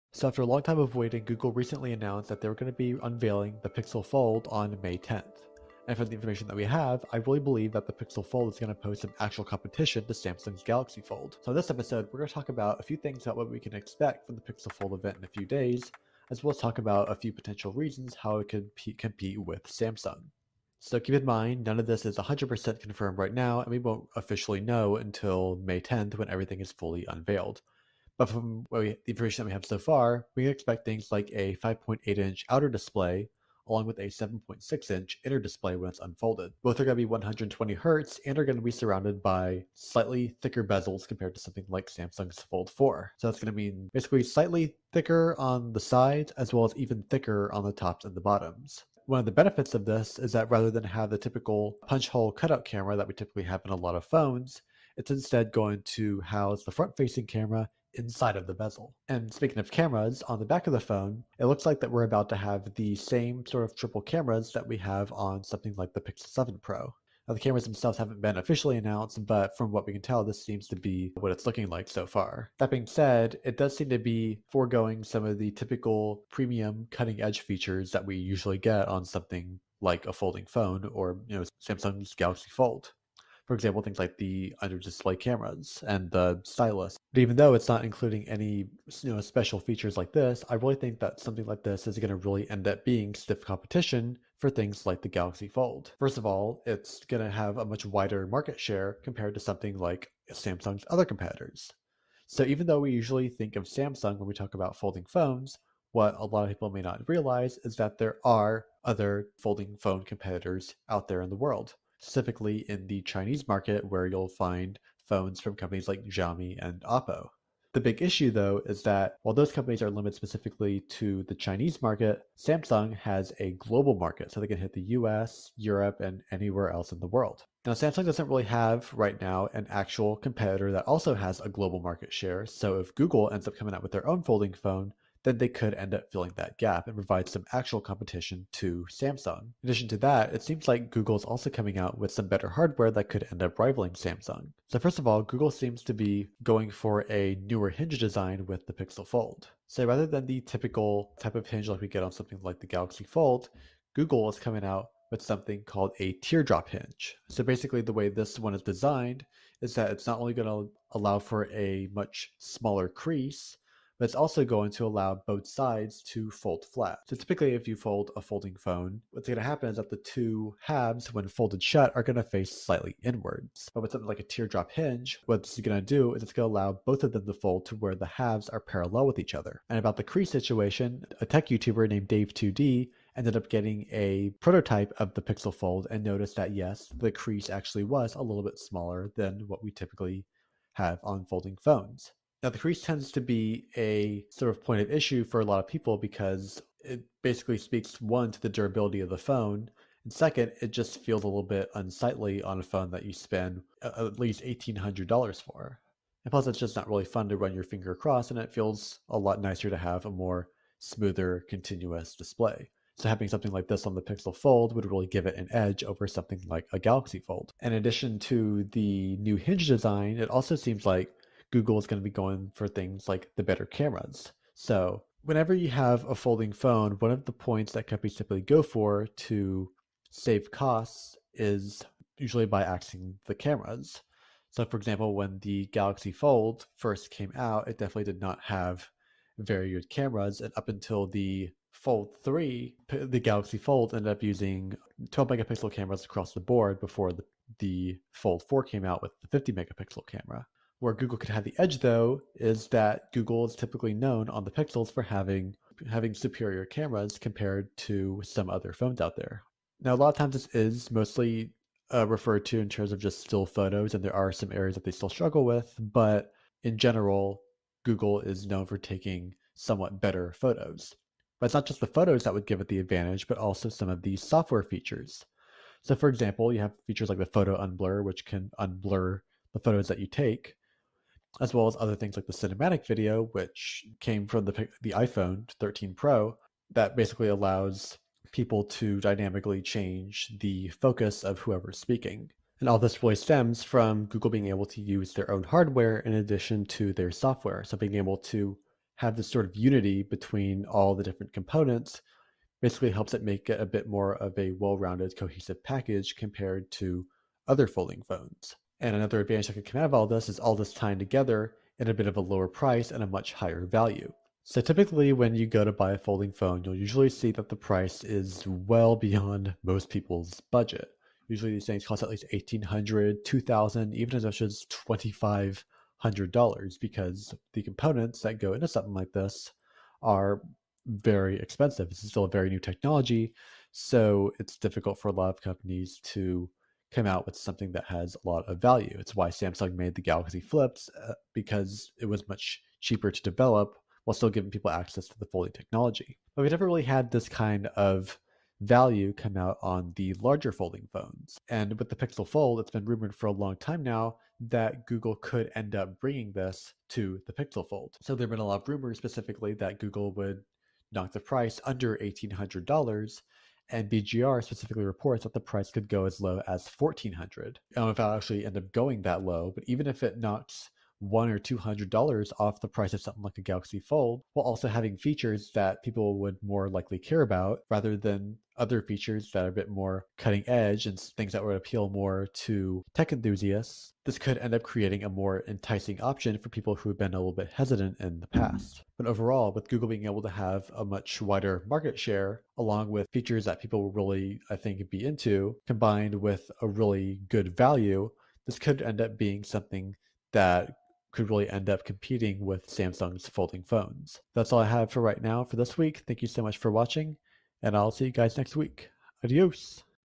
how pixel fold will compete with galaxy fold5
Pixel Fold is almost here! In this episode, we discuss some of the new features Google is expected to reveal for the Pixel Fold at Google I/O 2023 as well as how it could end up competing with Samsung’s Galaxy Fold and Galaxy Flip lineup…

⏳ // TIMESTAMPS
0:00 What to expect
1:26 How it could compete